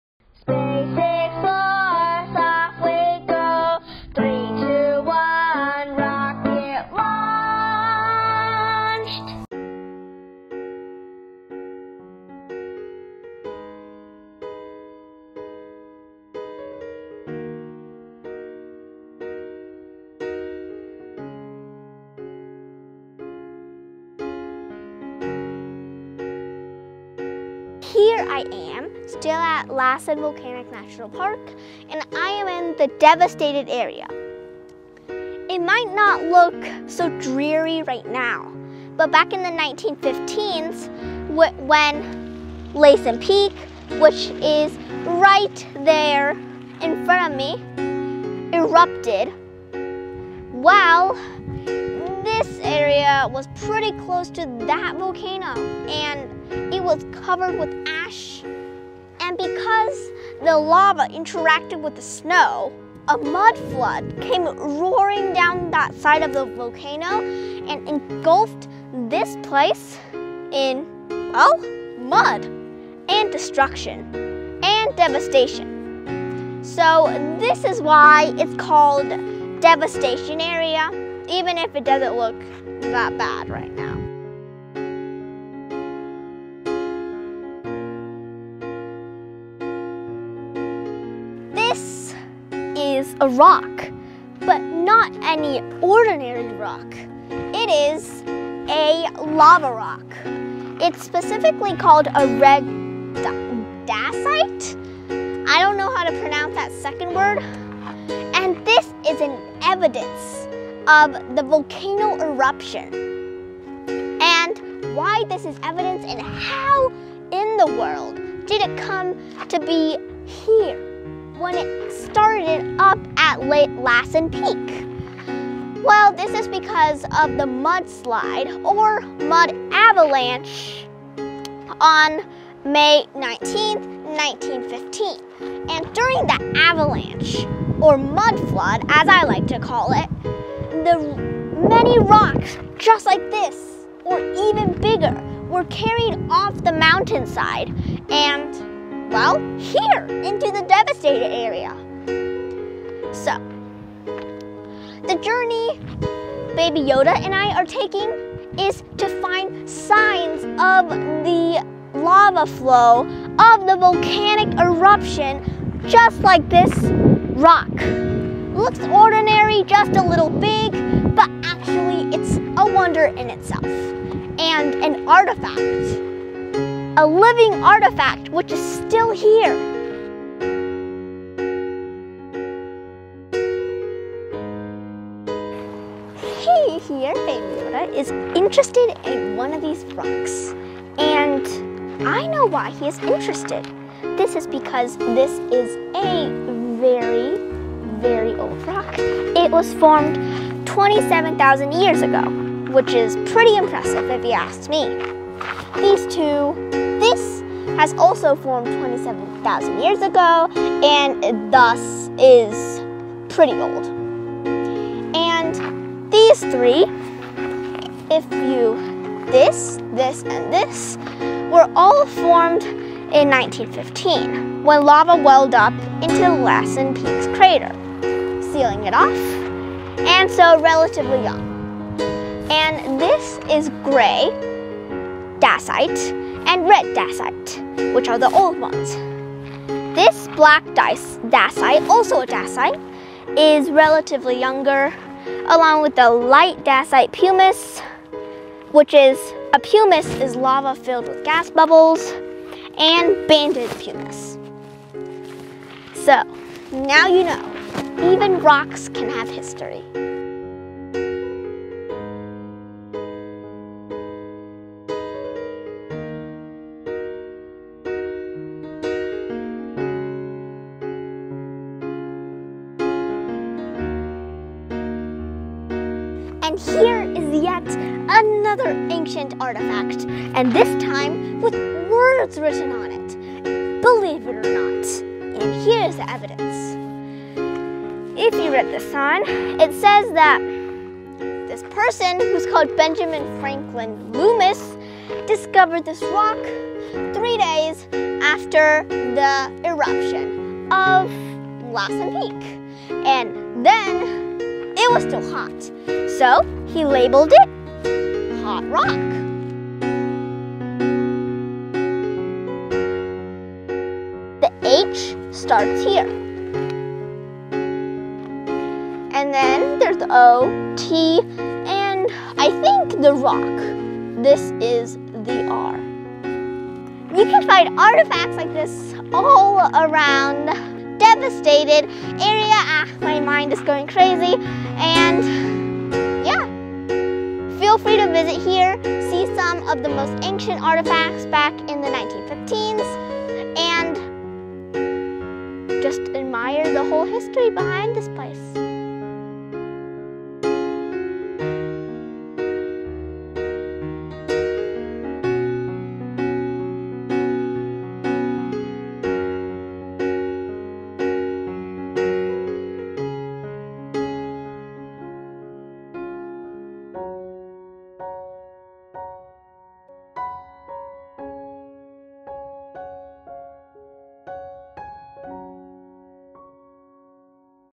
How old are these ancient rocks?| Lassen Volcanic National Park|Lassen Peak|Space Nina Show
Boom! Bang! Crash!
On May 19, 1915, Lassen Peak, the largest plug dome volcano in the world, located in Lassen Volcanic National Park, erupted, oozing miles and miles of thick, viscous, deadly-hot lava.
Tourists and civilians in the area stared, still as a rock, as they saw the rivers of crimson come towards them, like the claws of a sleepy red dragon. Then, before they knew it, mudflows were crashing down on them like rogue waves, a result of the melted ice and snow mixing in with the dirt and grime up at the peak and flowed down due to gravity's pull.
People screamed in terror and scattered before you could even say "Holy Hephaestus!" As they manuevered their way towards safety, people prayed that fallen rocks would not strike them dead on the spot.
They were lucky. No fatalities.
Afterwards, giant rocks from on top, mainly dacites and pumices (they hardened after hitting Earth's surface) crashed down from an elevation of 3,187.3 meters (10,457 feet). They rolled until the smoke no longer flew up in tufts from their surfaces, snuffed out by the cold grainy ground and lodged in place, unmoved and untouched (except for somebody engraving "Hot Rock" on one of its surfaces afterwards).
They are still here today, in the Devastated Area of Lassen Volcanic National Park.
Excited to learn more about these rocks with me, Space Nina, and my greatest ever Star Wars pal, Baby Yoda? Click start on this video and dive into this fascinating adventure. Travel with me to the year 1915 as we explore the very area (now not as devastated as it used to be) where the first hot rocks landed, flying down from 10,457 feet above.

Talk about a time capsule!